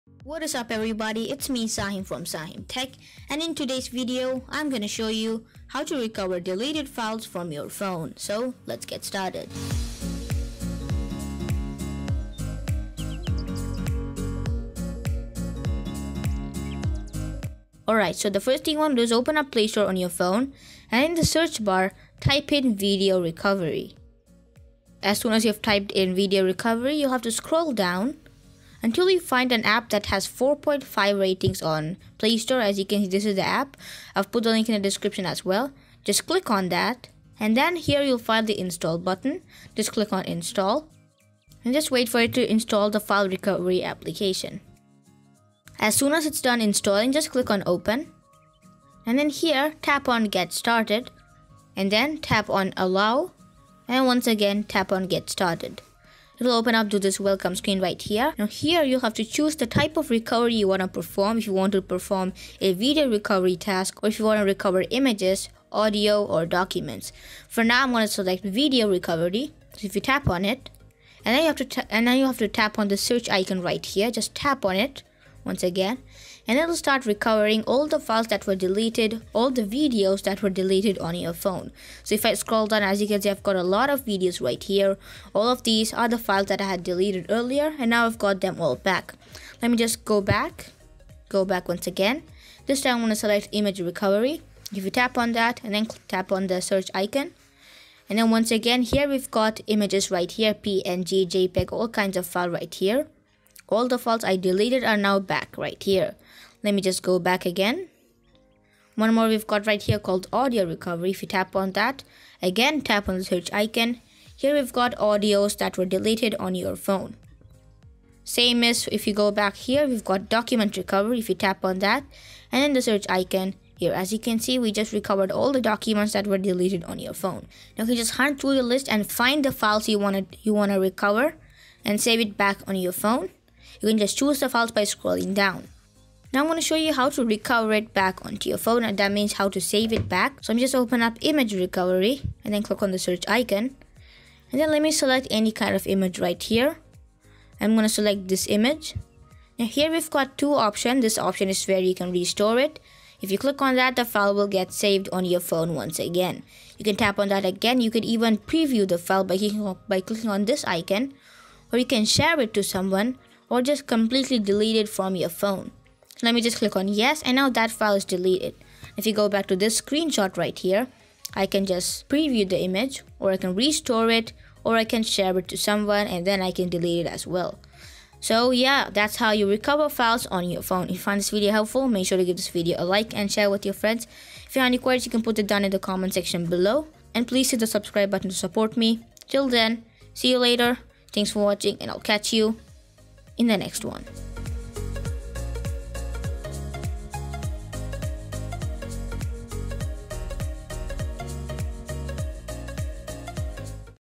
How to Recover Deleted files on your Phone!
This Video shows How to Recover files that we're once Deleted on your Phone!
Install the App:
⏳ Time Stamps:
0:00 Intro
0:17 Installing the App
0:46 Launch the App
0:51 Give Permissions
0:55 Types of Recovery
1:06 Video Recovery
1:32 Photo Recovery
1:50 Audio Recovery
1:57 Document Recovery
2:21 How to Restore it?
2:57 Outro
📝Any Queries or Suggestions? Fill out his form!:
Music Visualizer Credits:
┌┴─┴─┐-┘─┘ Peace Out
(SahimTech)